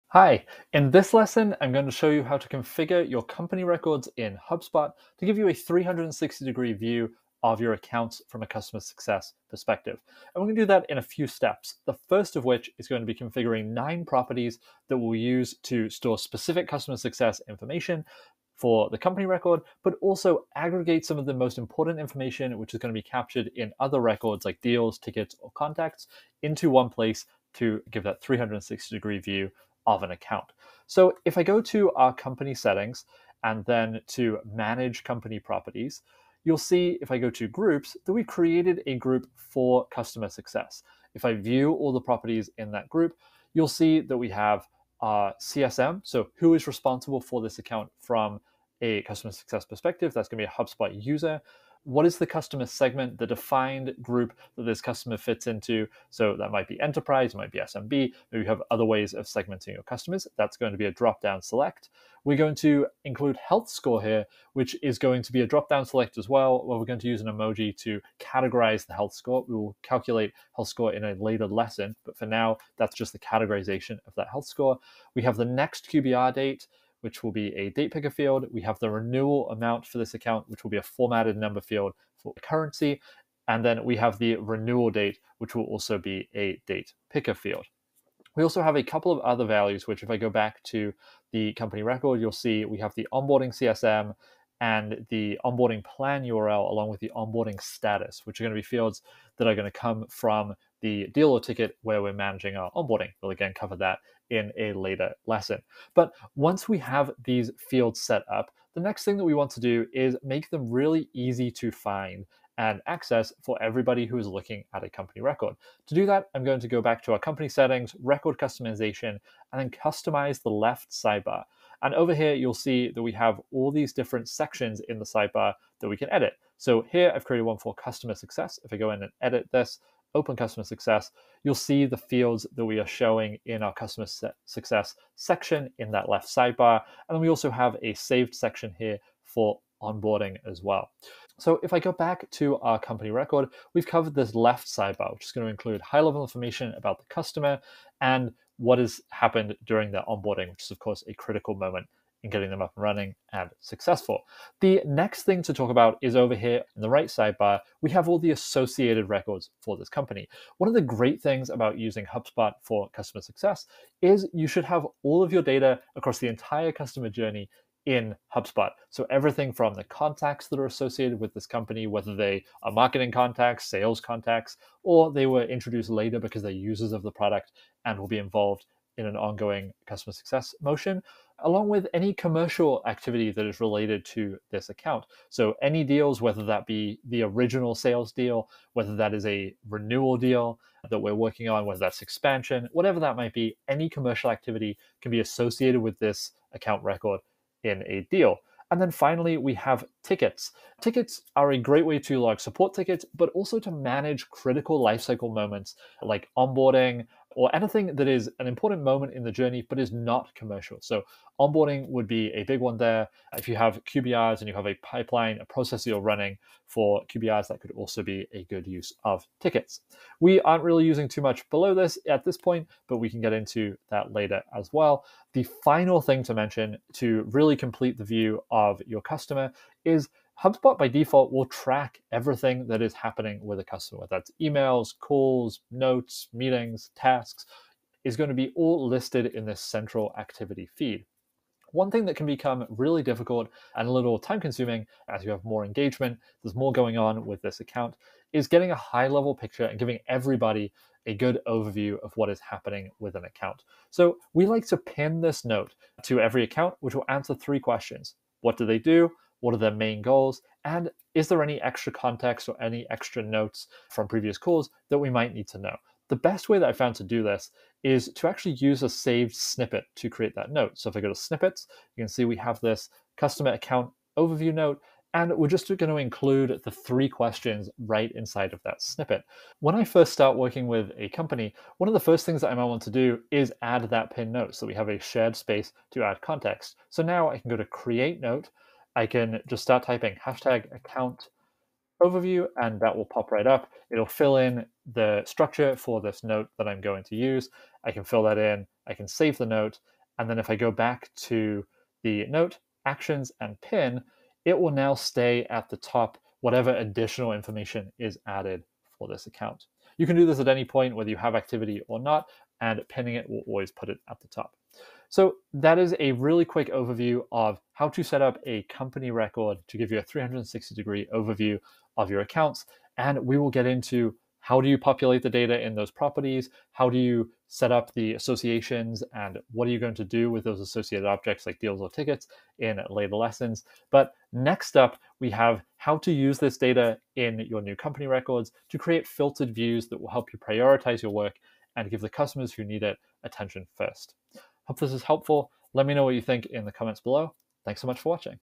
How to create 360-degree view of customer accounts in HubSpot
One downside of trying to use HubSpot for customer success is you need to do some configuration to get setup to enable your team.

The good news…it’s not as hard as you might think to give your team what they need to get a complete picture of their accounts and make their customers successful.

Here’s how to centralize account data by being intentional about how you use HubSpot company records:

1. Add a saved section in the left sidebar for your most important customer success information:
→ Renewal Date
→ Next QBR Date
→ Customer Health Score
→ CSM

2. Add another section in the left sidebar for high level information about onboarding:
→ Onboarding Specialist (who was responsible)
→ Onboarding Plan URL
→ Onboarding Status (Complete, In Progress, Failed)

3. Add a pinned note that includes a high level account summary and answers these 3 questions:
→ What do they do?
→ What are their goals/success metrics?
→ What notable context must everyone know?

4. Associate “contacts” who have been engaged from this account.
Associate “deals” for each commercial activity in the lifecycle.
→ Initial sales
→ Renewals
→ Upgrades, expansions, contractions

5. Associate “tickets” that have been created for this account.
→ Onboarding
→ Support

Bringing all your account data together in one place not only makes it easier for everyone who interacts with the customer to quickly gain context and be better prepared to make them successful…

…but it also enables improvements to internal workflows and reporting that allow your team to focus their time on their highest leverage work.